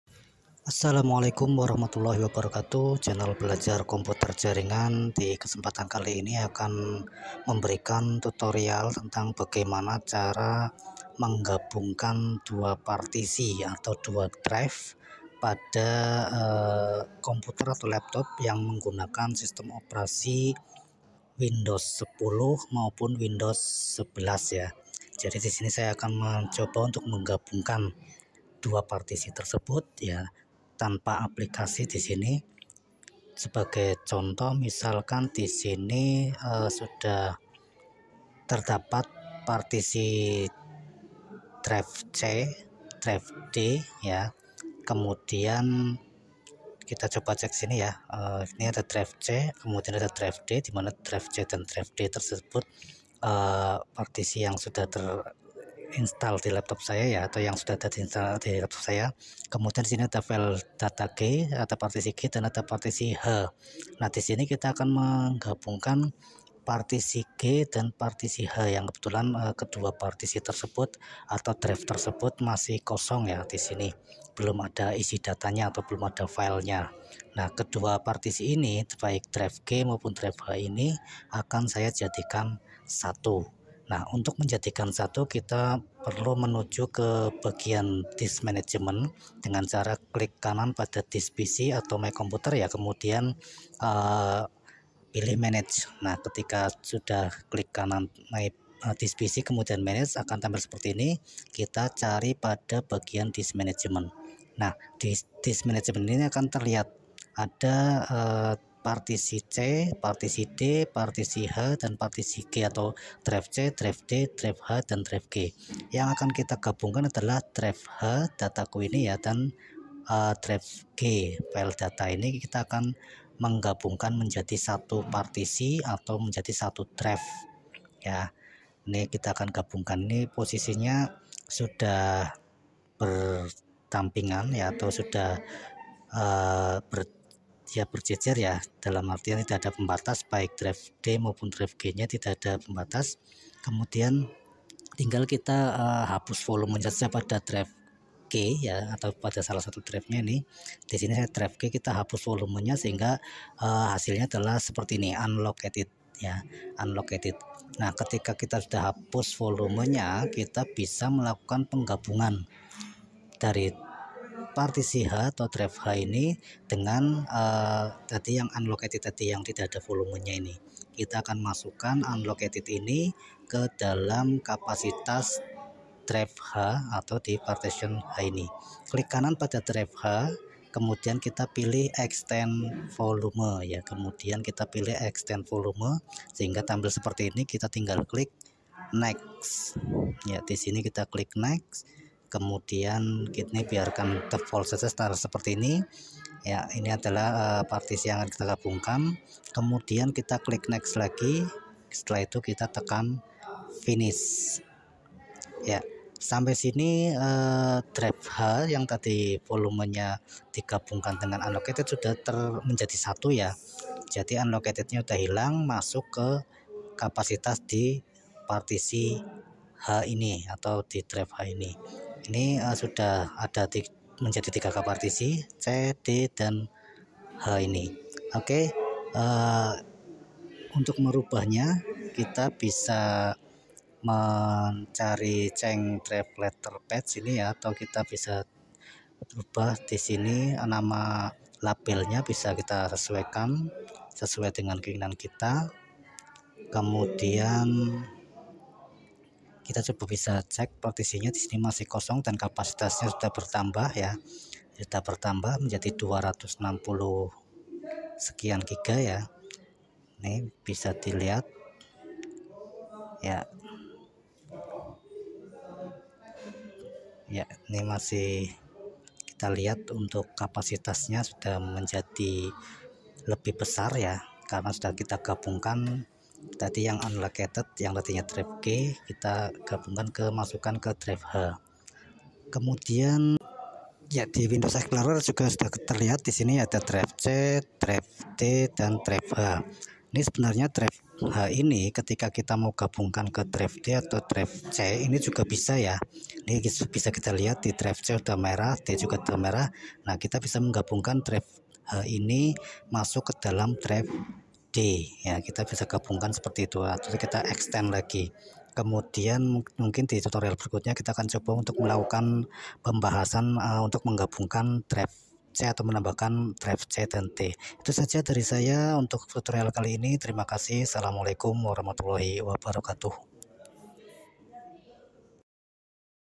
Cara Menggabungkan Drive Partisi Hardisk Di Windows 10 dan Windwos 11 Tanpa Aplikasi
Berikut ini adalah video tutorial tentang bagaimana Cara Menggabungkan Drive Partisi Hardisk Di Windows 10 dan Windwos 11 Tanpa Aplikasi.
Drive atau partisi hard disk merupakan bagian dari media penyimpanan di dalam hard disk yang digunakan untuk menyimpan data atau file di laptop atau komputer. Hard disk didalam laptop atau komputer dapat dibagi menjadi beberapa partisi, biasanya terdapat partisi C atau drive C yang digunakan untuk menyimpan data sistem operasi laptop atau komputer, partisi D atau Drive D yang digunakan untuk menyimpan data atau file kita seperti foto,video atau berkas/dokumen word, pdf dan lain-lain.
Untuk menggabungkan dua partisi atau drive menjadi satu dapat menggunakan aplikasi tambahan atau tanpa aplikasi.
Di video tutorial Cara Menggabungkan Drive Partisi Hardisk Di Windows 10 dan Windwos 11 Tanpa Aplikasi ini fokus yang akan dibahas adalah bagaimana cara menggabungkan atau menyatukan drive atau partisi didalam hard disk laptop atau komputer tanpa menggunakan aplikasi tambahan dengan tujuan menggbungkan dua partisi atau drive ini adalah agar kapasitas partisi atau drive menjadi bertambah besar.
Bagaimana Cara Menggabungkan Drive Partisi Hardisk Di Windows 10 dan Windwos 11 Tanpa Aplikasi silakan simak saja langsung video tutorial Cara Menggabungkan Drive Partisi Hardisk Di Windows 10 dan Windwos 11 Tanpa Aplikasi ini.